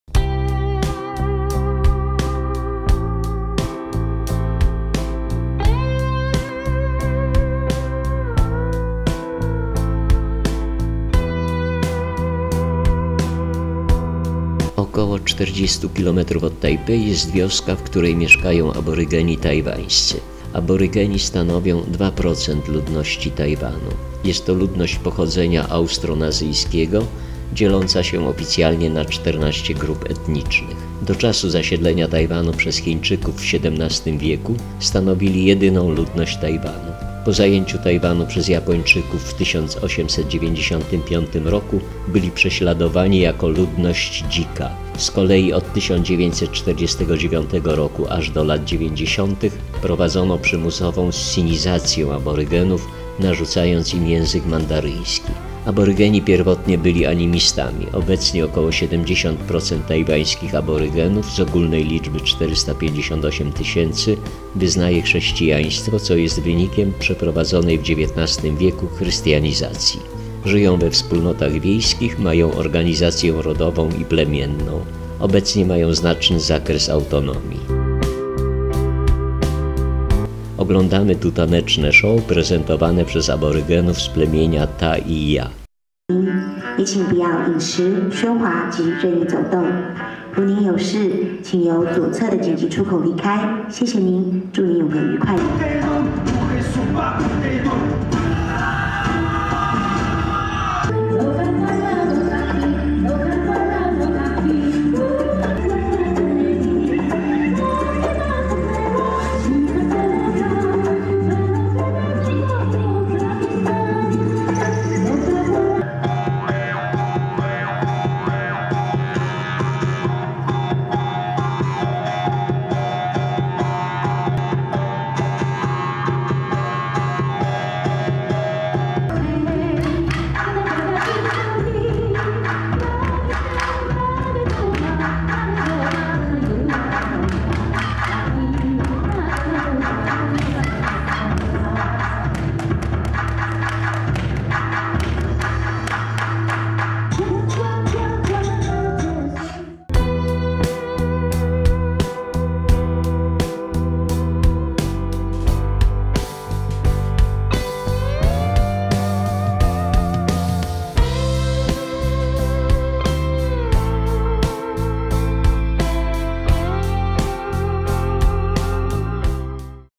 Tajwan - Aborygeni tajwańscy (Wioska Wulai)
Aborygeni stanowią 2% ludności Tajwanu. Oficjalnie dzielą się na 14 grup etnicznych. Do czasu zasiedlenia Tajwanu przez Chińczyków w XVII w. stanowili jedyną ludność Tajwanu.